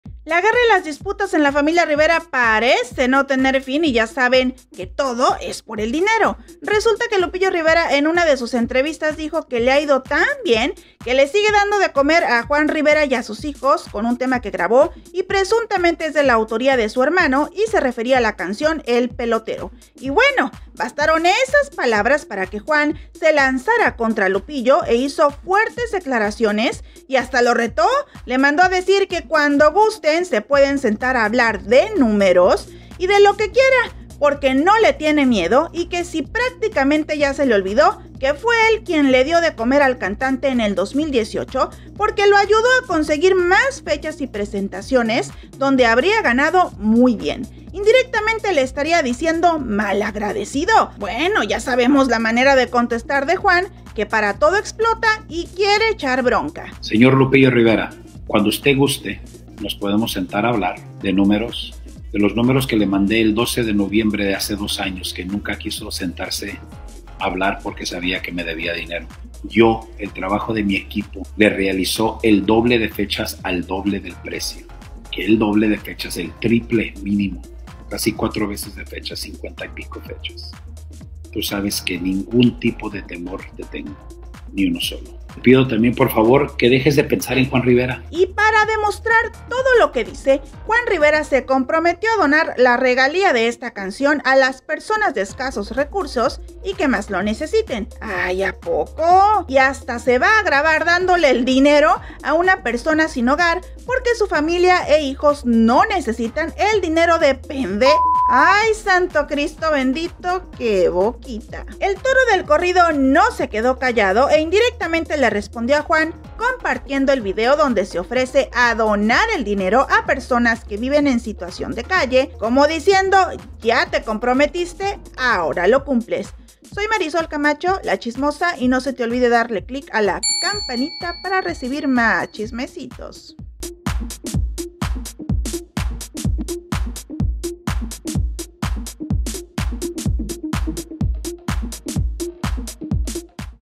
🔵 Juan Rivera RETA a Lupillo Rivera❗ Dice que NO LE DA DE COMER Y QUE LE HIZO GANAR MUCHO DINERO😱😱
JuanRivera enfurece contra LupilloRivera porque el Toro del Corrido dice que le ha dado de comer con una sola canción.